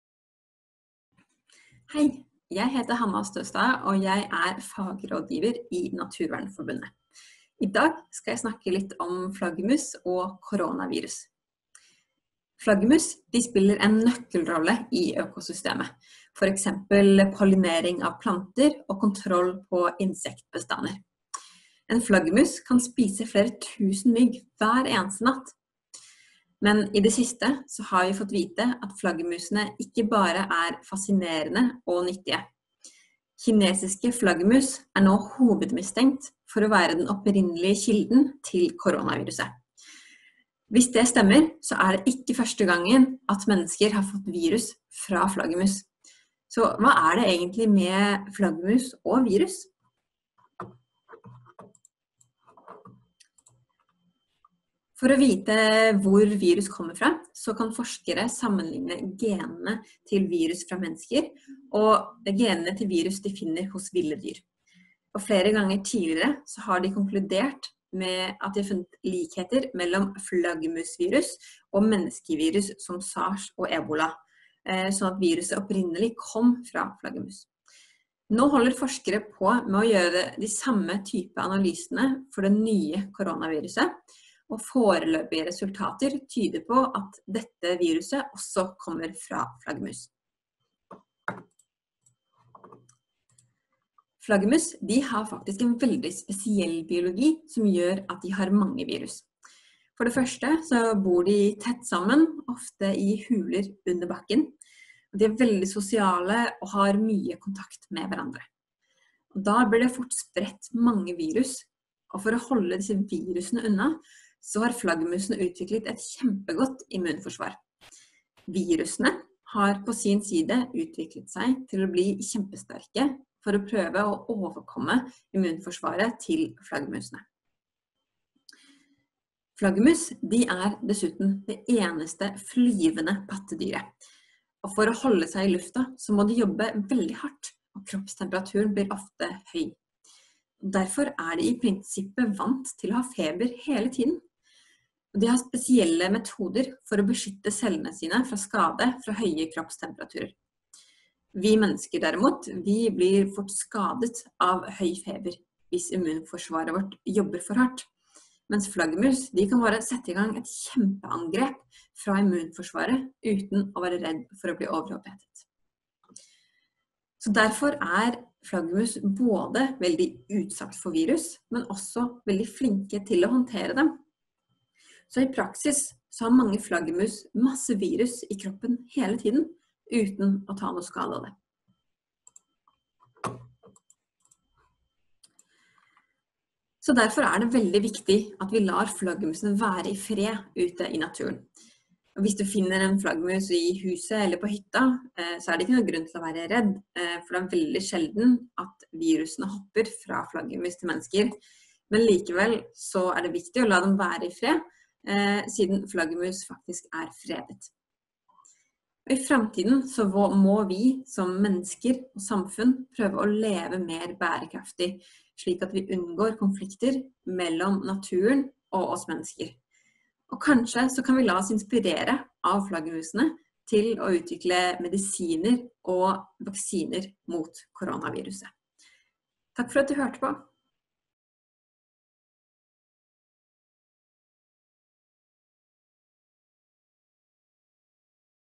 Flaggermus og Korona
Hvorfor tror man at Korona-virus kommer fra flaggermus? Er dette første gangen det har skjedd? Hva er det som gjør at flaggermus utvikler virus som kan være farlig for oss mennesker? Dette og mer forklares av Hanna Nyborg Støstad, fagrådgiver i Naturvernforbundet.

En rekke virusepidemier som har plaget menneskeheten er antatt å stamme fra flaggermus, inkludert SARS og Ebola. Forskere har analysert genene til SARS-CoV-2, og funnet mange likheter med koronavirus fra kinesiske flaggermus. Det er også funnet et liknende virus hos pangoliner, og SARS-CoV-2 har muligens oppstått i flaggermus og så blitt overført til en pangolin og videre til mennesker. På grunn av flaggermusenes biologi er sannsynligheten likevel stor for at viruset opprinnelig dukket opp hos dem. Det har skjedd før, og det vil sannsynligvis skje igjen.
La meg forklare hvorfor.